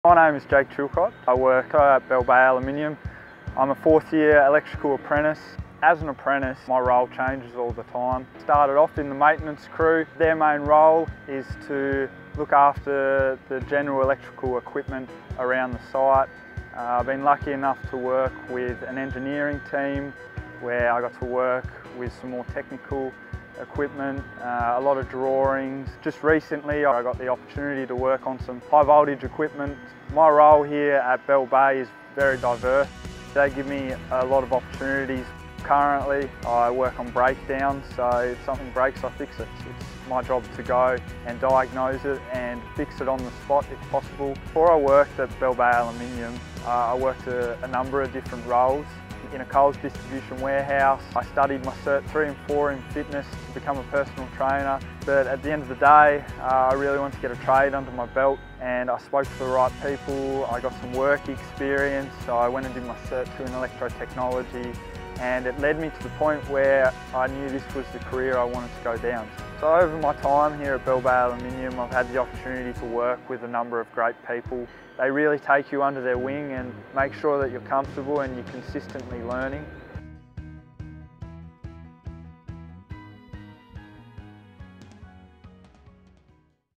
Electrician in the Tasmanian Minerals Sector | TMEC
The Tasmanian mineral resource industry are global leaders in environmental care, community, human rights and safety and generates 64% of Tasmania's export value from just 1% of our land area.

Meet, Jake, Apprentice Electrician at Bell Bay Aluminium.
“Something I’ve learned over my years here. It’s all about attitude.” - Jake.

Our critical minerals are in high demand for new tech including electric vehicles, renewables, healthcare and the technology we all use every day.

It’s also critical that we source these minerals with contemporary land practices, minimising our carbon footprint with environmental sustainability at the core of everything we do.

There are thousands of diverse roles to be filled in the next 5 years. Help lead the world to a better future.

Cleaner. Smarter. Diverse.